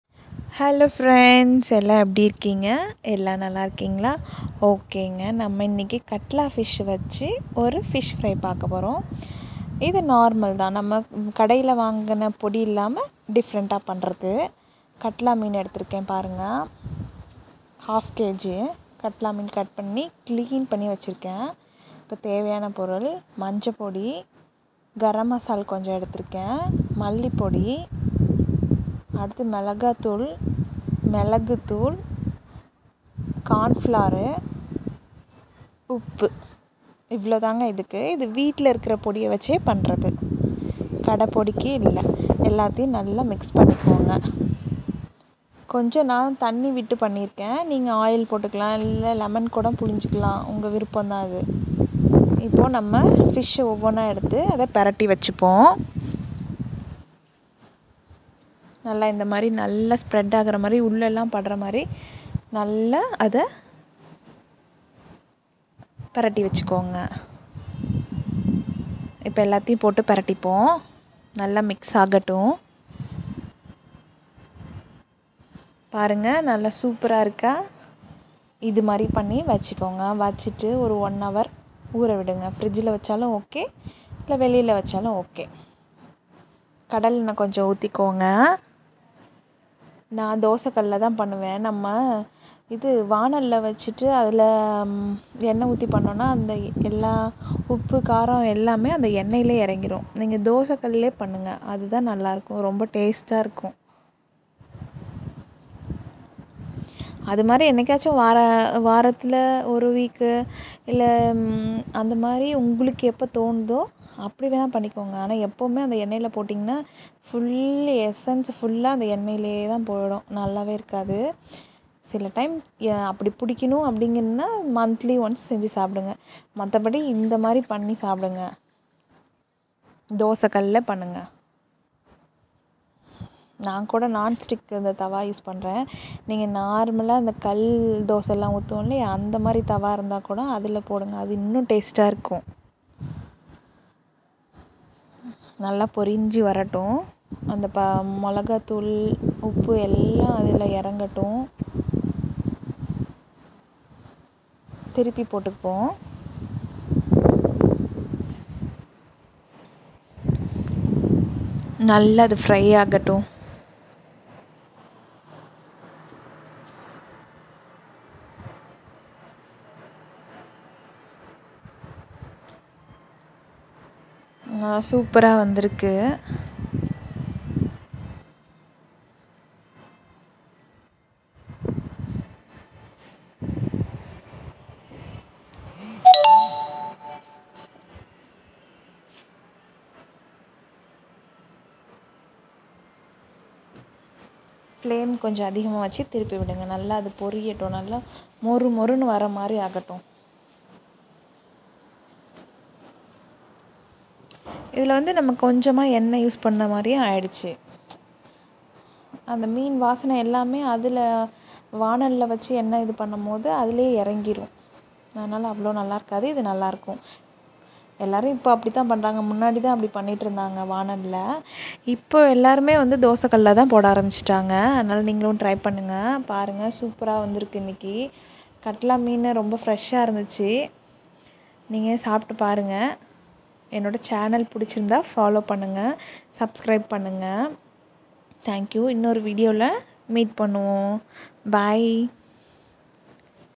katla meen varuval in tamil
try pani parunga friends.simple masala fish fry